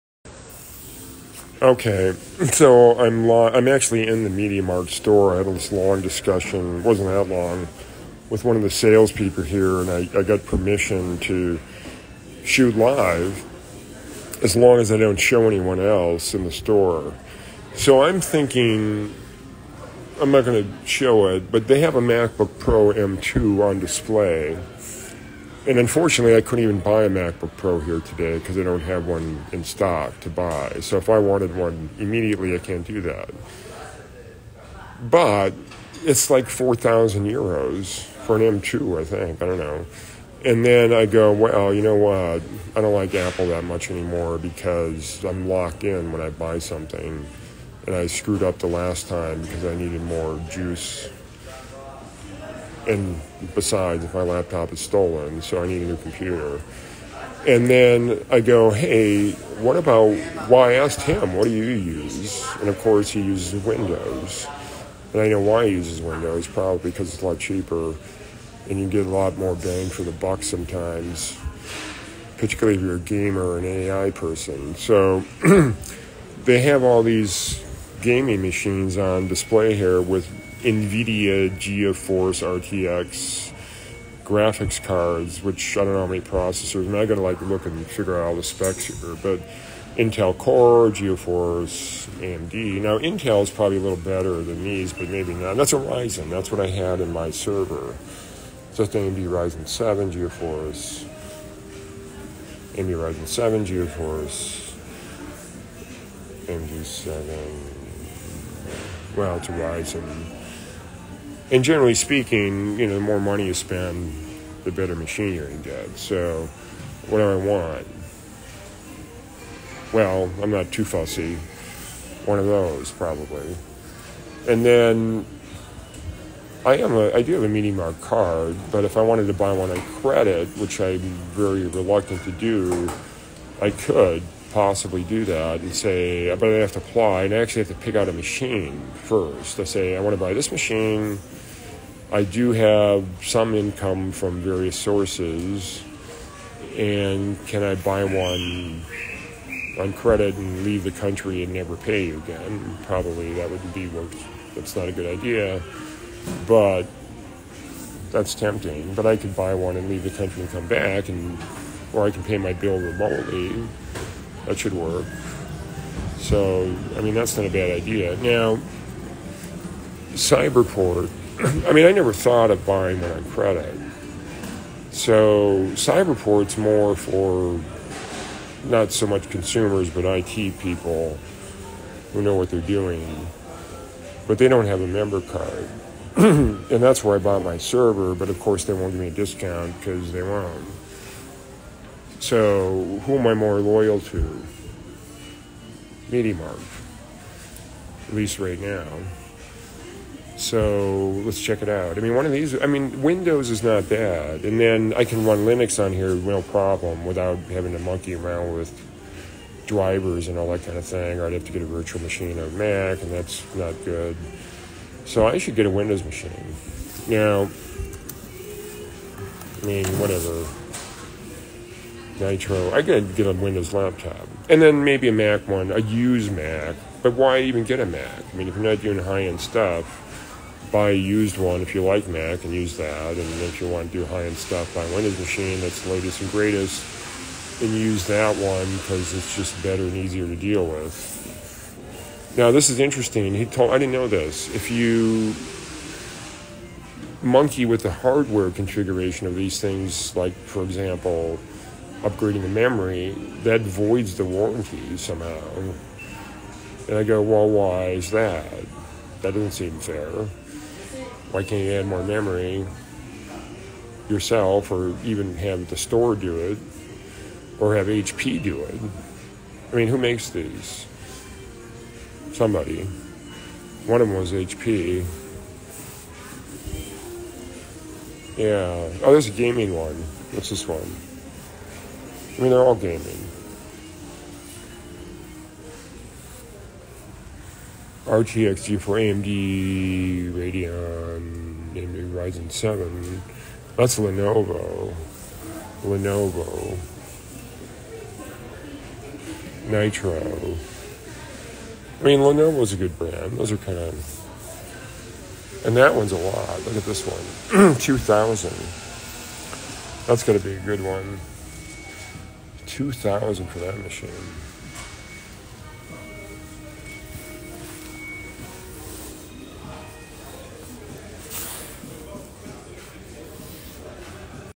Software development Laptop Computer options.
If I wanted to replace a MacBook Pro M1 chip mac with 16GB RAM with a Windows Machine what should I pick ?  I sort of want one with an NVIDIA card, but I think it is possible to hook up an NVIDIA card to a Mac using a USB cable, or I could get a Mac Pro with PCI slots, but those are really expensive.